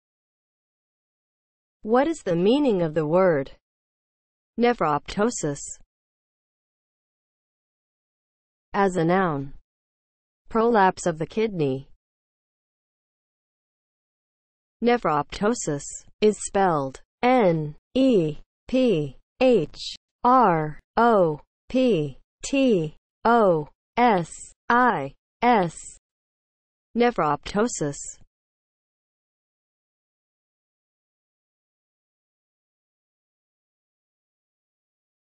What is the meaning of the word NEPHROPTOSIS?
Definition and spelling of the word NEPHROPTOSIS.

(00:00) Intro
(00:07) As a noun
(00:13) Spelling

As a noun (00:07):
• prolapse of the kidney